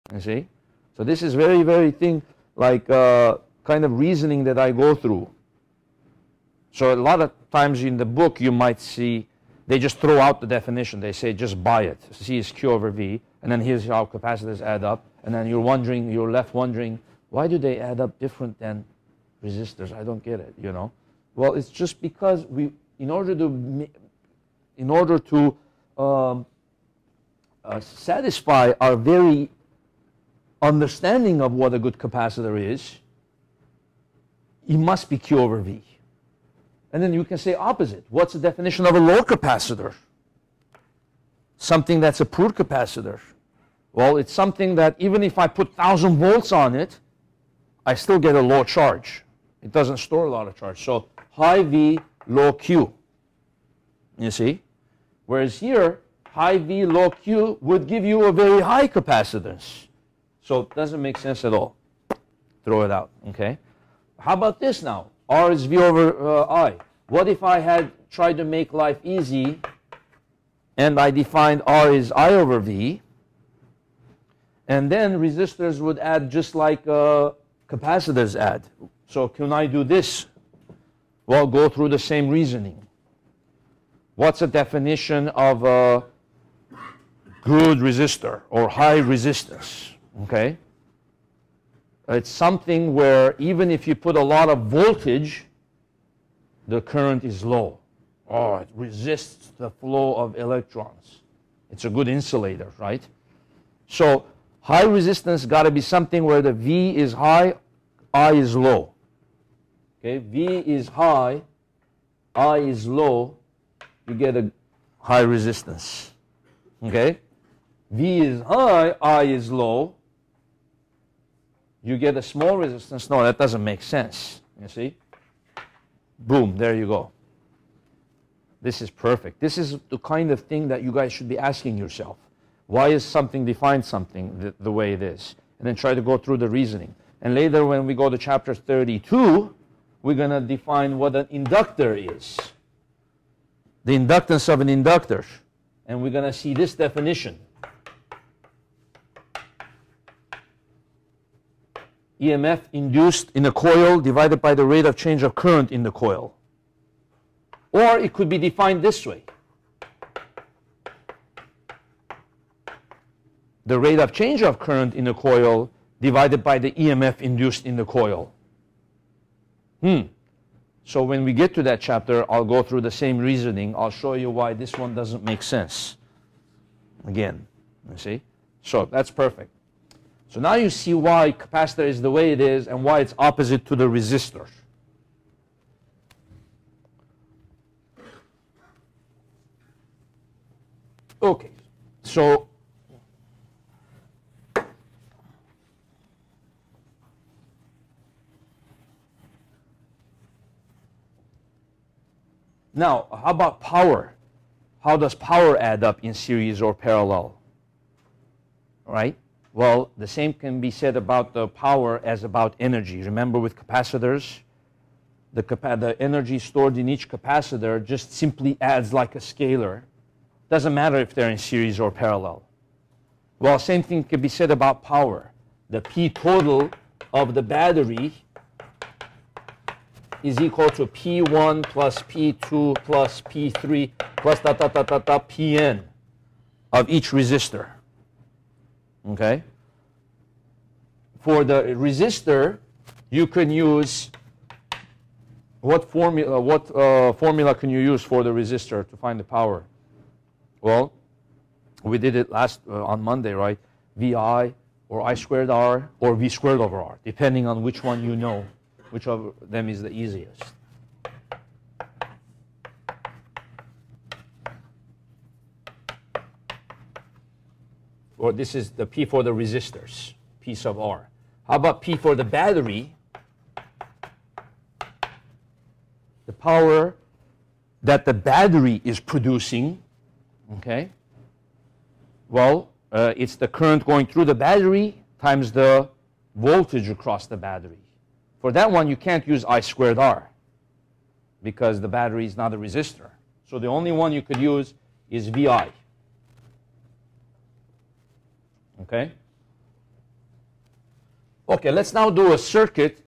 Resistors and Circuits Theory Part 2
This clip gives the definition of series and parallel circuits and shows the derivation of how resistors add up in series and parallel.  (Part 2)